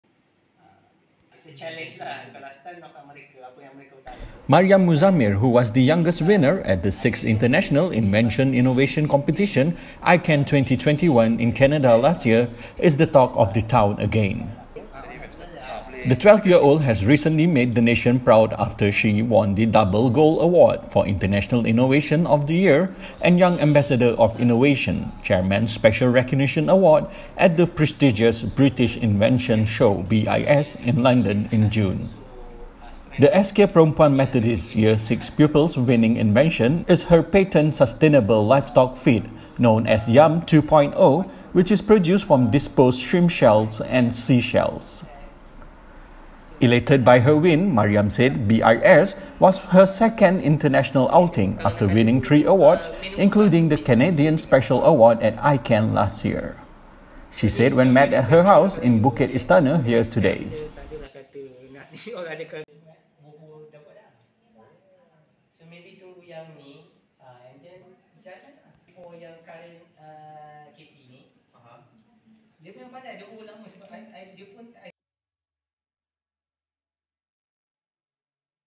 PAHANG: Young inventor Maryam wins two awards at British innovation competition
Maryam Muzamir, who was the youngest winner at the 6th International Invention Innovation Competition (iCAN 2021) in Canada last year, is the talk of the town again.

The 12-year-old has recently made the nation proud after she won the Double Gold award for International Innovation of the year (industrial) and Young Ambassador of Innovation - Chairmans Special Recognition Award at the prestigious British Invention Show (BIS) in London in June.

The SK Perempuan Methodist Year Six pupil's winning invention is her patented sustainable livestock feed known as Yam 2.0 which is produced from disposed shrimp shells and seashells.

Elated by her win, Maryam said BIS was her second international outing after winning three awards, including the Canadian Special Award at iCAN last year.

She said when met at her house in Bukit Istana here today.